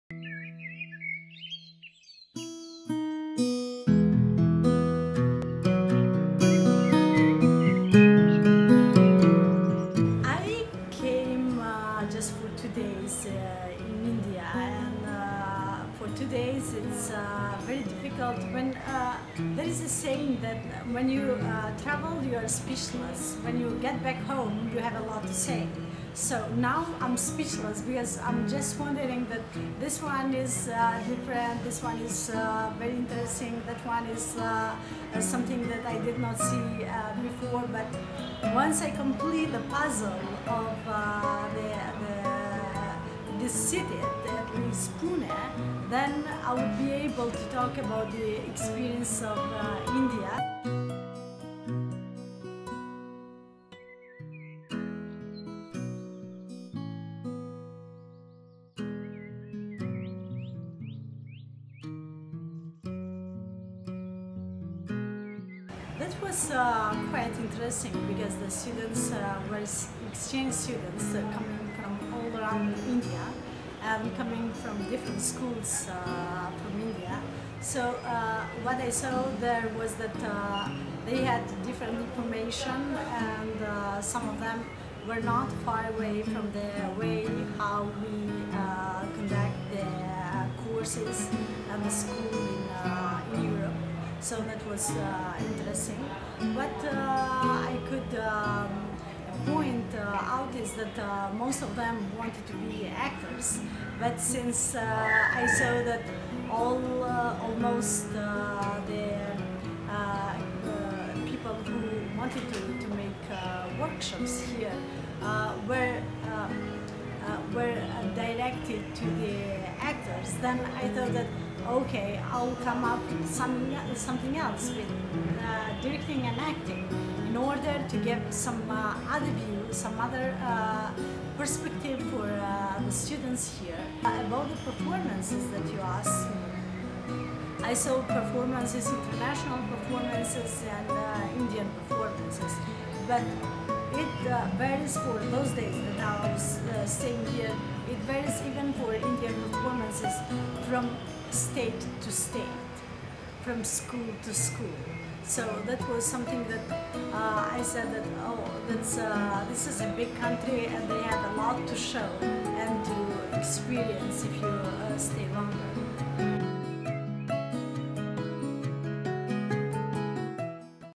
IITF 2017 - In Conversation with Elmaze Nura (Kosovo)
Throwback to IITF 2017! Senior Theatre practitioner from Kosovo Elmaze Nura, speaks about the festival.

IAPAR International Theatre Festival 2018 will be held in Pune between 11th to the 18th of November 2018.